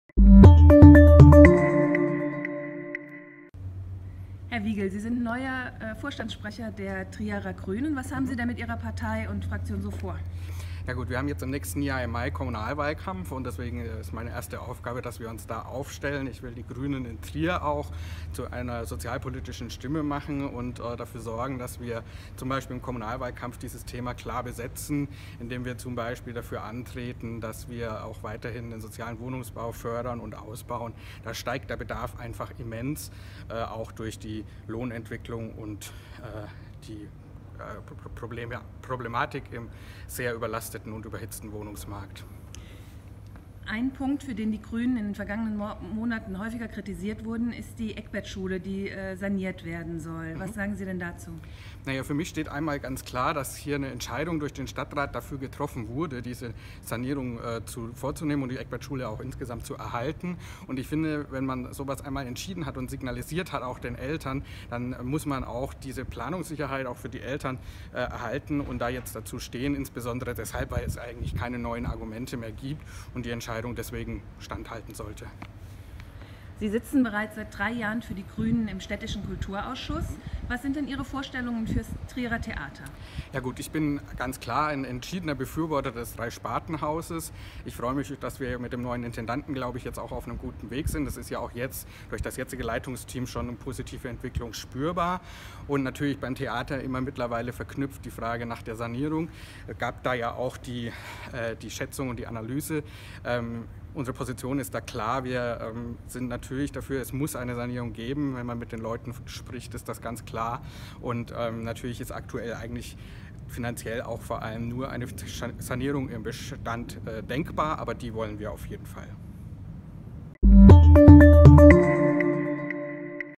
Johannes Wiegel ist neuer Chef der Trierer Grünen
Solo statt Duo an der Parteispitze: Für den zweiten Sprecherposten, der laut Satzung mit einer Frau besetzt werden muss, gibt es keine Bewerberin. Nun muss der 26-jährige Wiegel alleine ran.